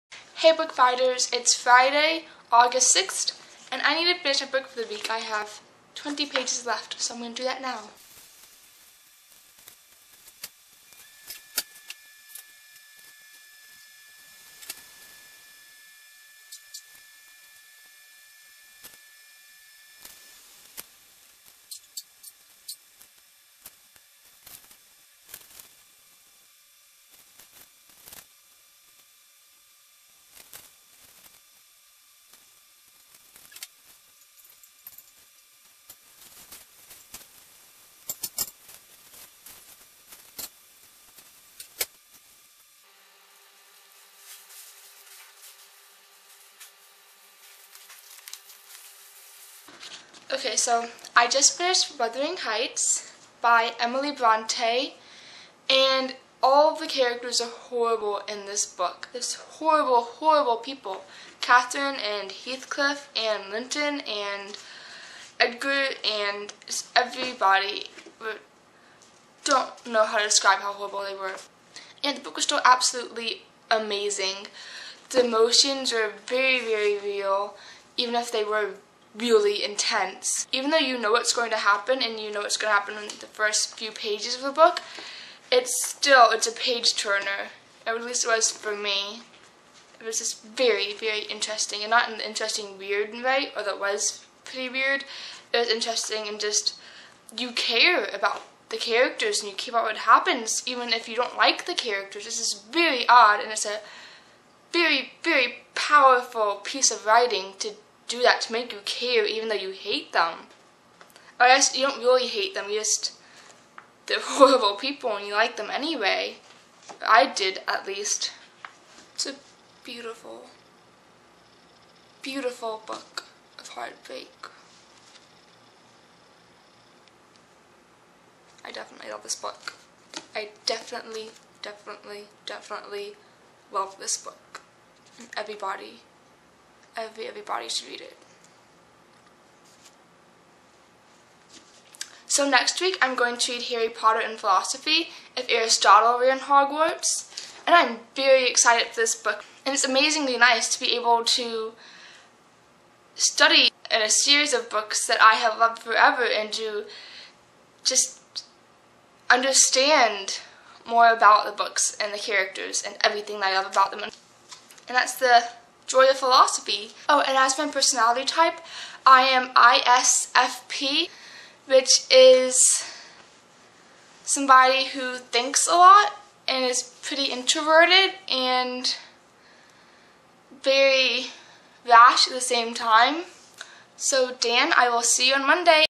Julia's review of Wuthering Heights- August 6th
This week I read Wuthering Heights, the classic tale of heartbreak, hardness, and general misfortune.  Next week I will read Harry Potter and Philosophy: If Aristotle Ran Hogwarts